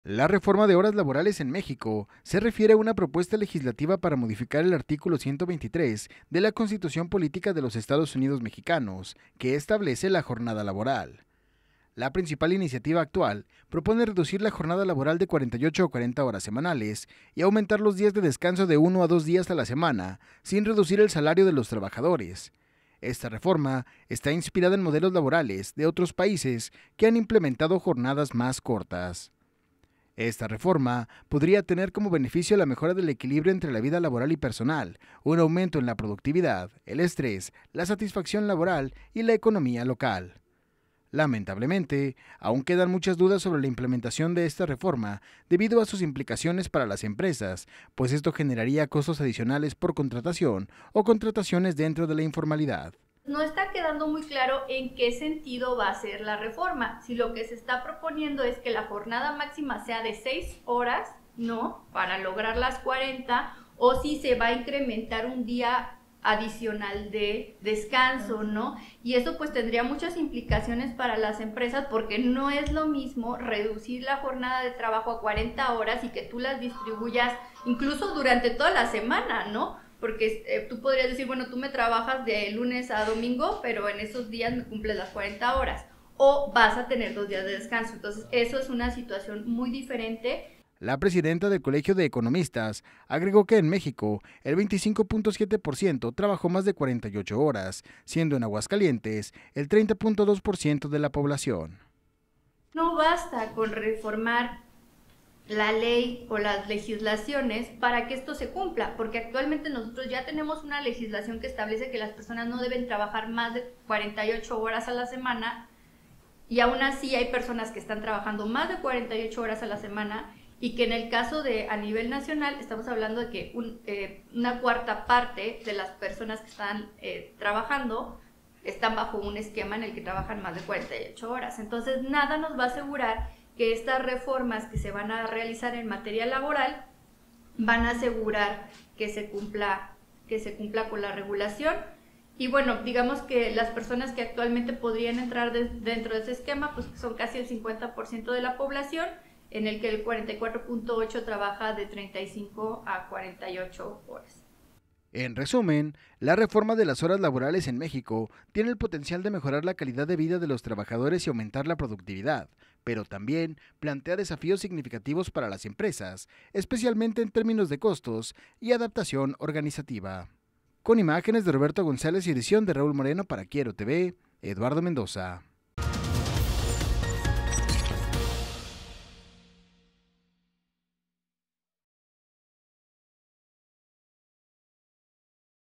Hasta más de 48 horas semanales trabajan algunas personas
📊 En Aguascalientes, el 30.2% de la población trabaja más de 48 horas semanales, según INEGI. 💼